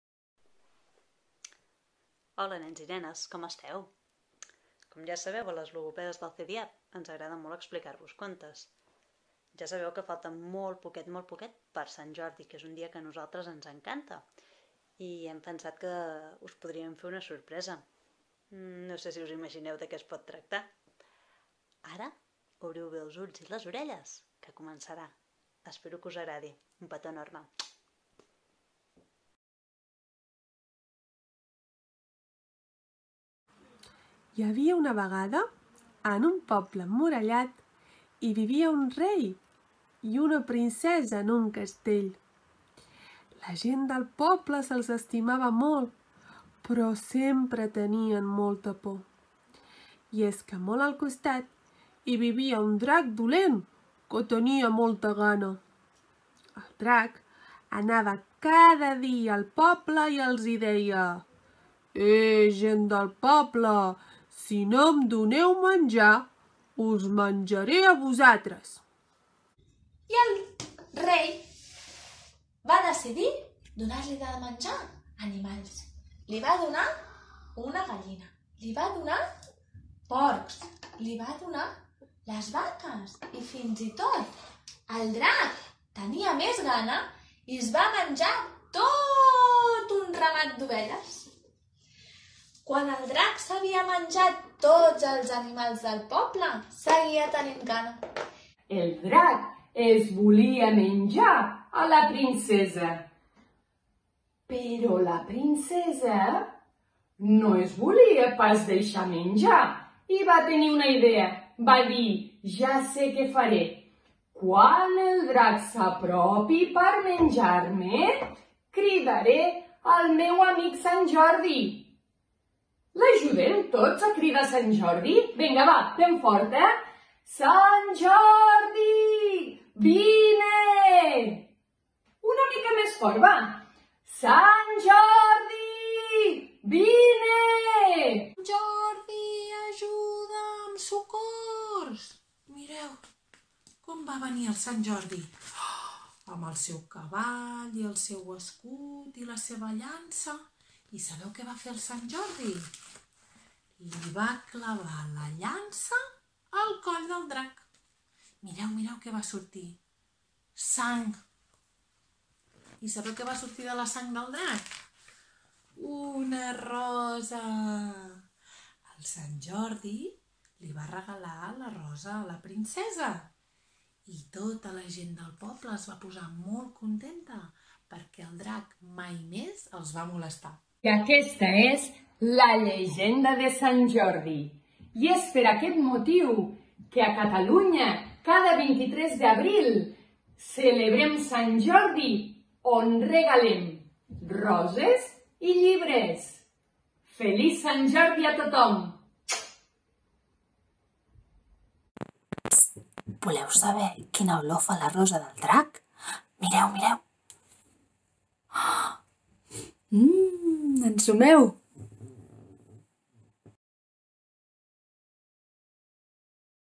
Expliquem la llegenda de Sant Jordi des del CDIAP del Parc Taulí
És sabut que La llegenda de Sant Jordi és un dels contes preferits de tots els nostres nens i nenes.

En aquest vídeo us mostrem diferents maneres d’explicar-lo utilitzant diversos materials de què podem disposar a casa.

El nostre desig és que agafeu idees i us ho passeu d’allò més bé explicant el conte amb els vostres fills i filles.

Centre de Desenvolupament i Atenció Precoç del Parc Taulí
Sabadell (Barcelona)